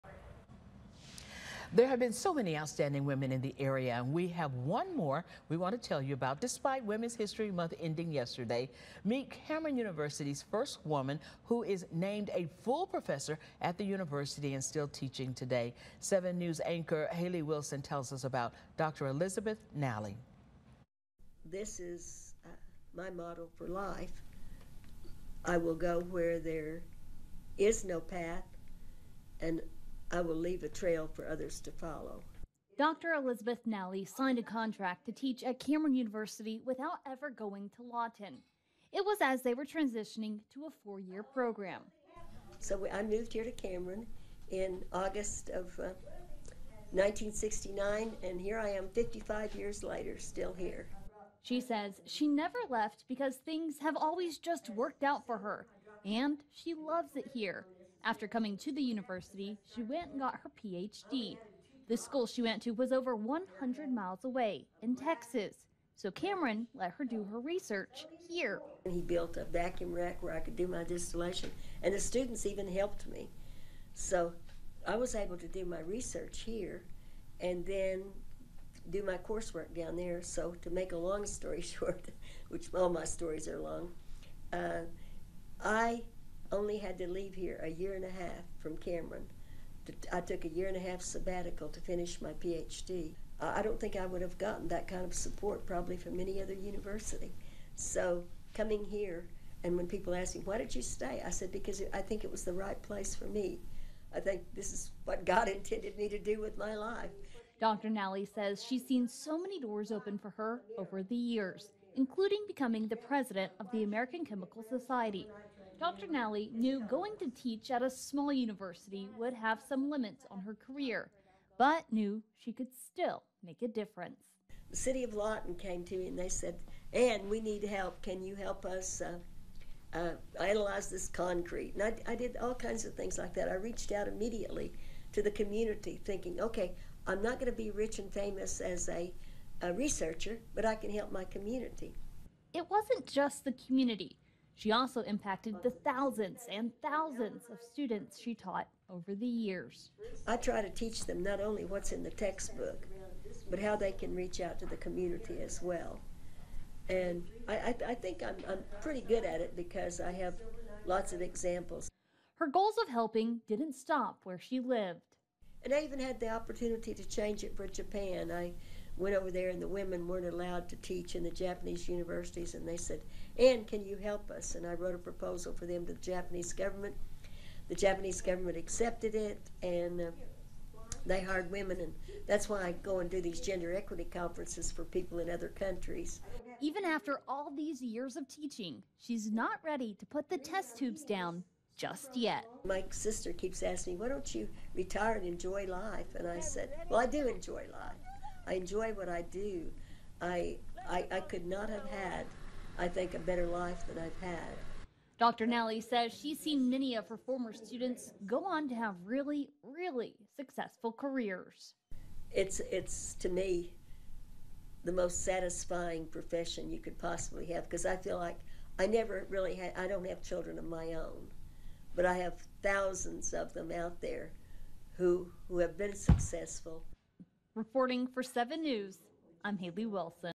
First woman to be full professor at Cameron reflects on teaching journey
Dr. Elizabeth Nalley signed a contract to teach at Cameron University without ever going to Lawton.